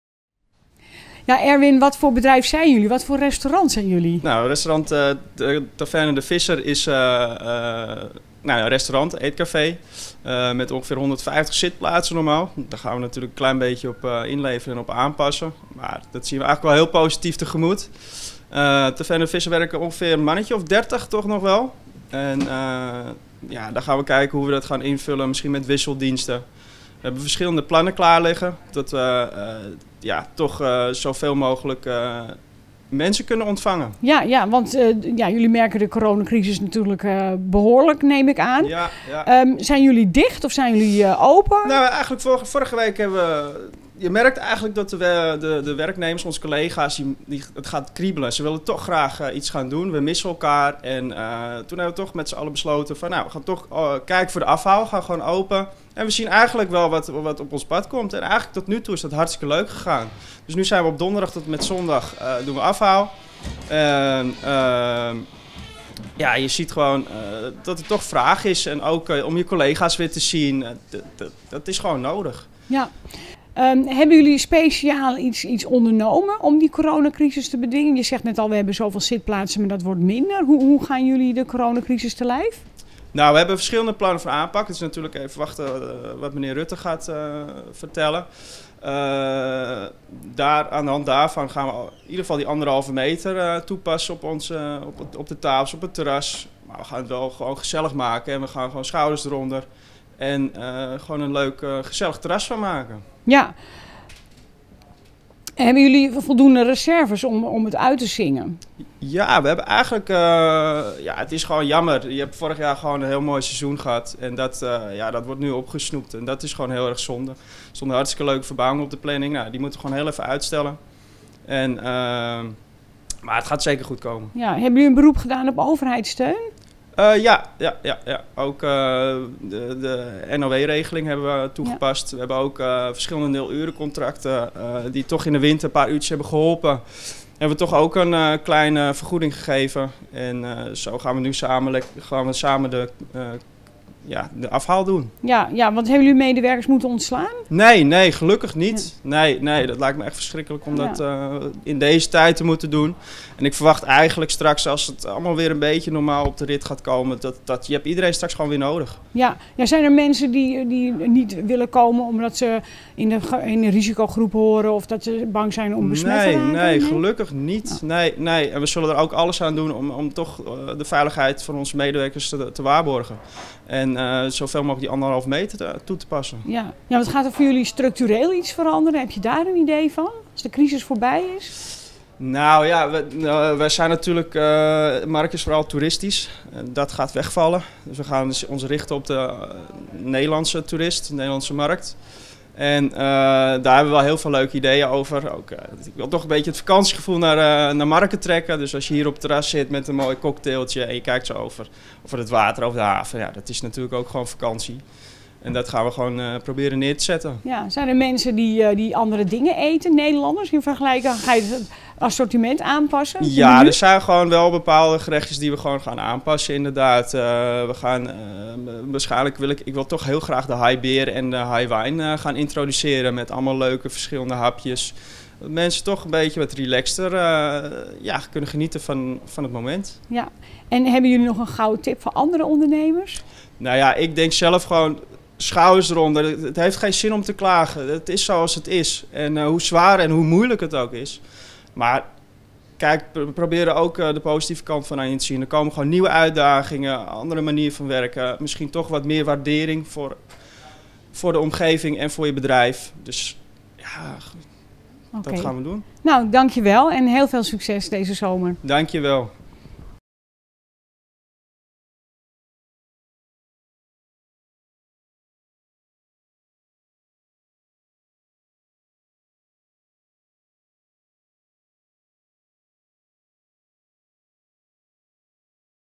Eetcafe De Visscher - Zo houden ondernemers hun hoofd boven Waterland
In de serie 'Zo houden ondernemers hun hoofd boven Waterland' sprak Omroep PIM met Erwin van der Meulen. Bij de Visscher is het stil, zonder de toeristen die normaal gesproken komen eten. Maar Erwin hoopt dat met de boot toch Nederlandse toeristen komen.